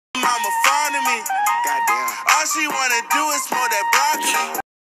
Parrot Doesn't Like Broccoli
White bird refuses broccoli